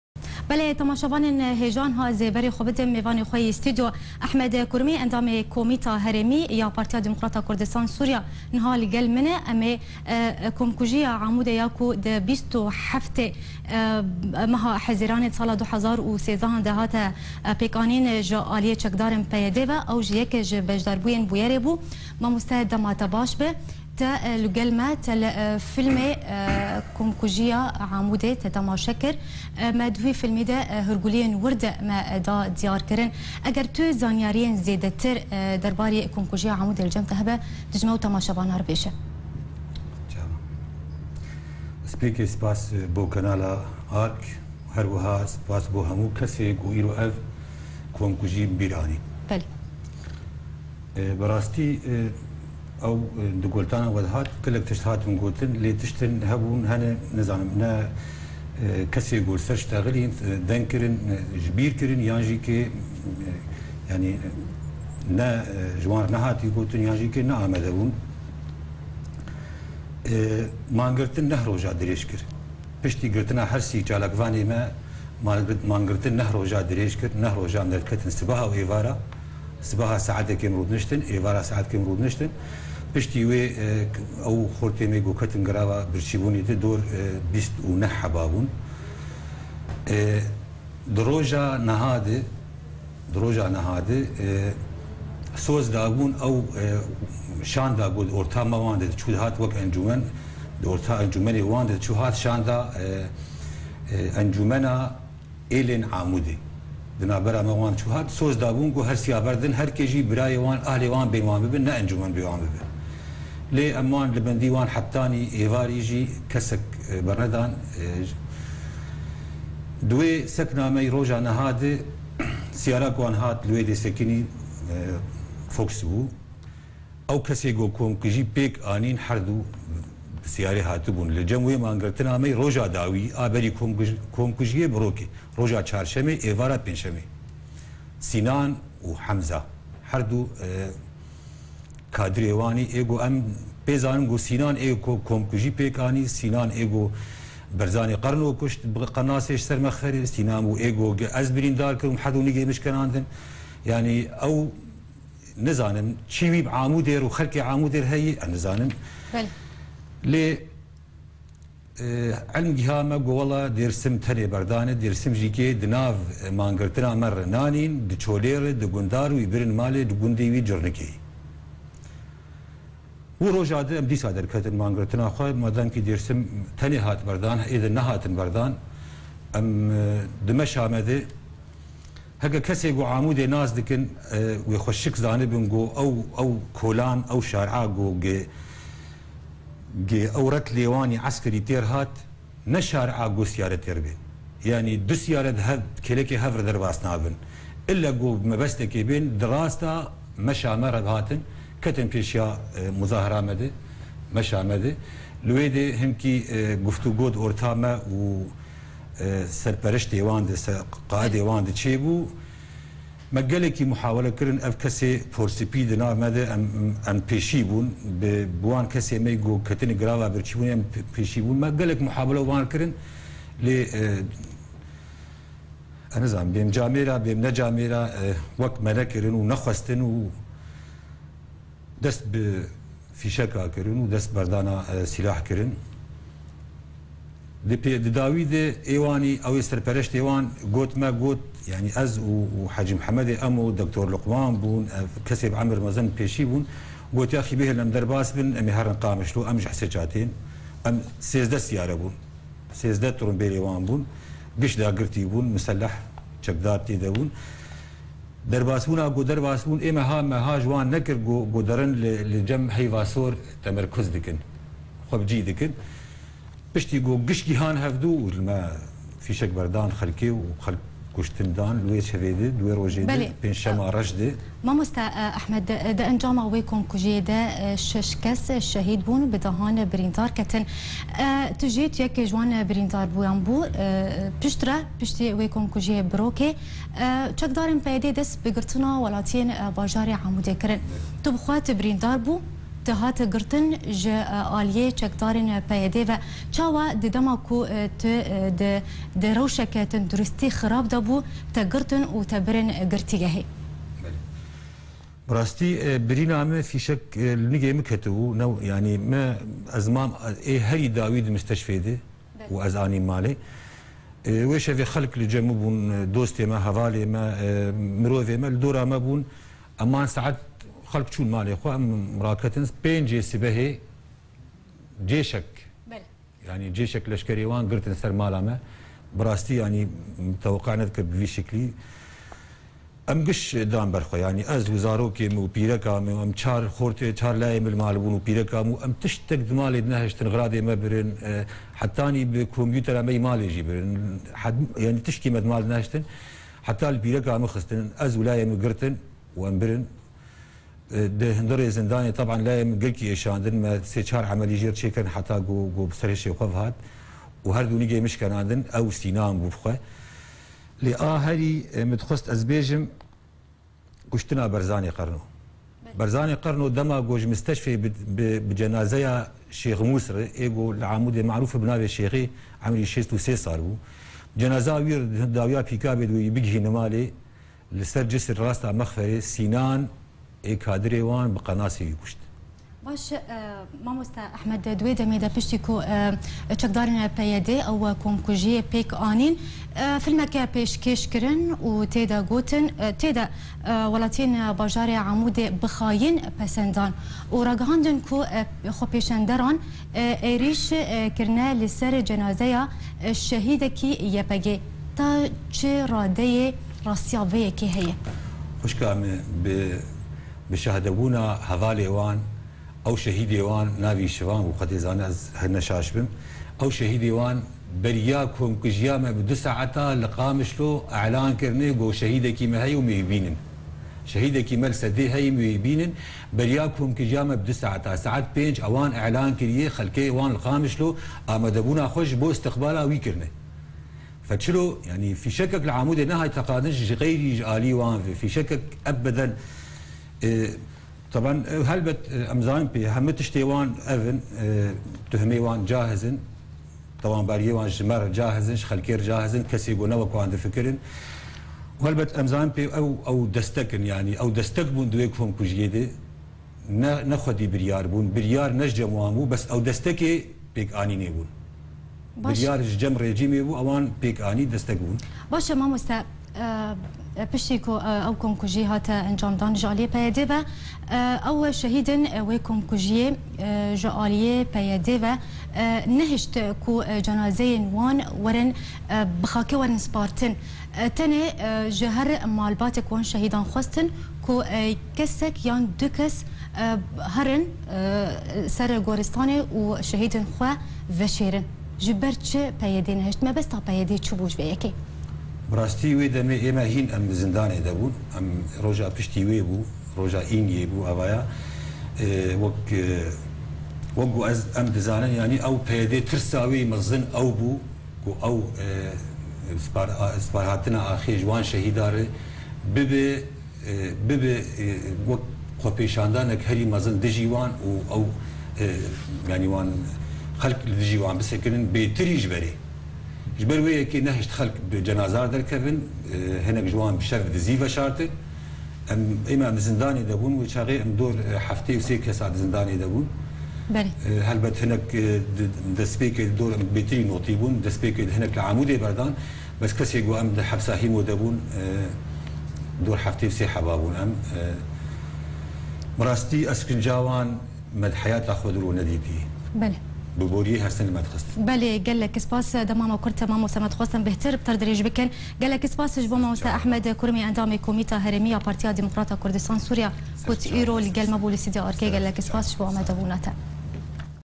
Ehmed Kurimî.. Endamê komîta herêmî yê PDK-Sê
3 Sal li ser komkujiya Amûdê re derbas dibin